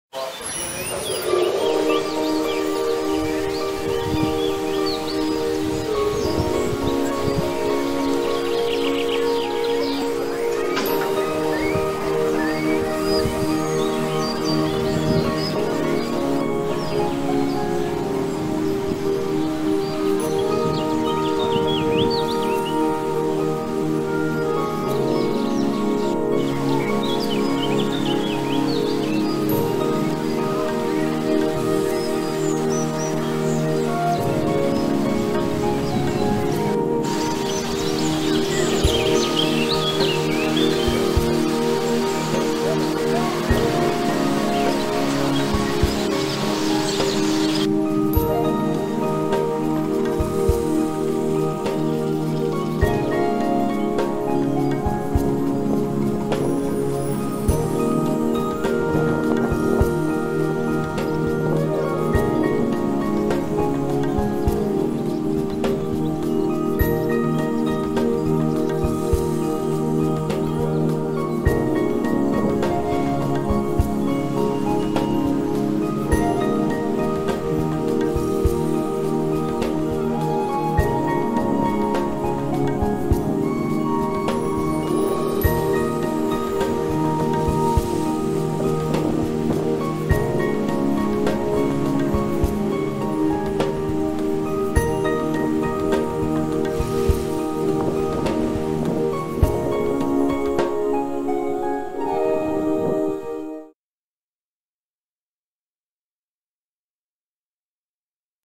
U Kopačkom ritu otvoreno turističko pristanište Sakadaš s pontonskim sustavom
U Kopačkom ritu otvoreno turističko pristanište Sakadaš s pontonskim sustavom koji se prilagođava vodostaju područja te osiguravanje bolje dostupnosti posjetiteljskim sadržajima u Parku prirode i omogučuje pristup osobama s invaliditetom.